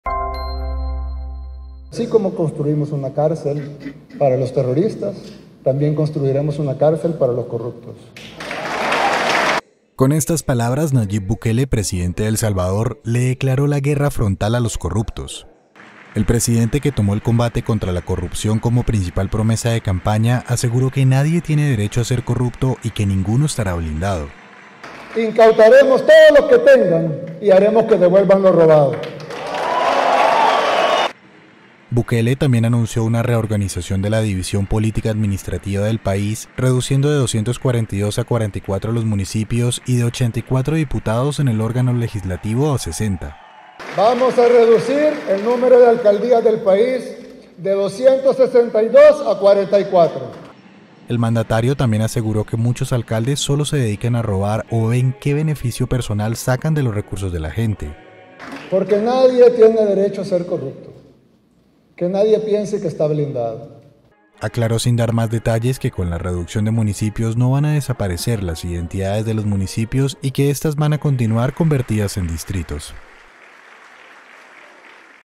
Nayib Bukele declara una "guerra frontal" contra la corrupción en El Salvador
El presidente de El Salvador, Nayib Bukele, anunció la noche de este jueves que su Gobierno llevará a cabo una "guerra frontal" contra la corrupción y señaló que se construirá una cárcel para "los corruptos".